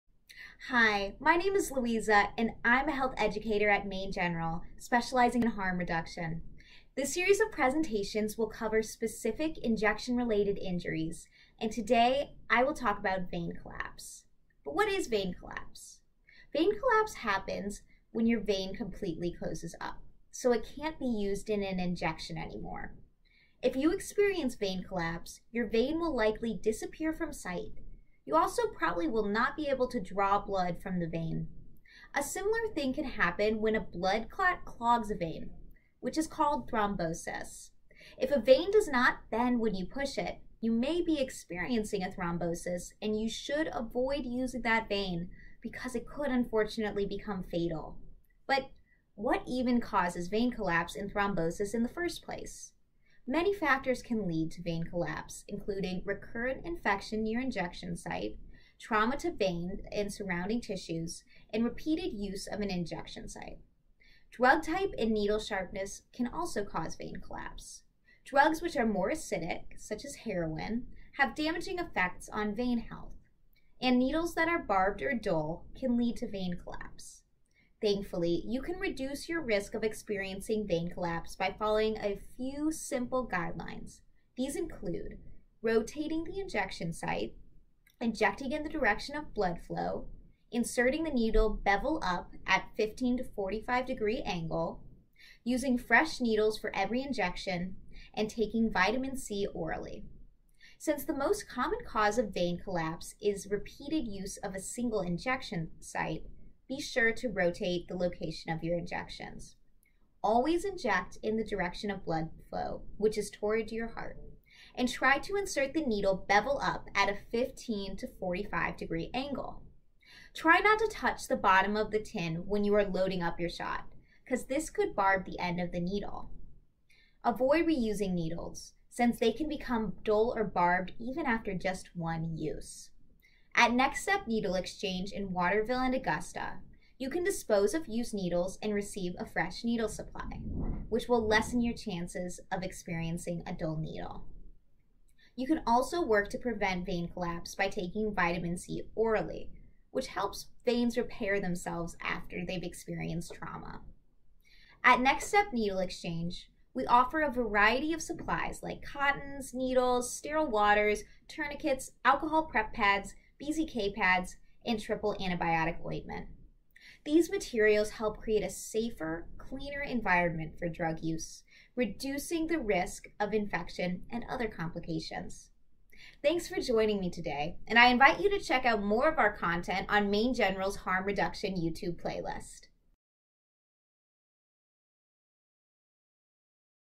Injection Related Injuries: Vein Collapse
This presentation covers vein collapse, a potentially dangerous injury which can result from injection drug use. The causes and health risks associated with vein collapse are discussed and methods of prevention are provided.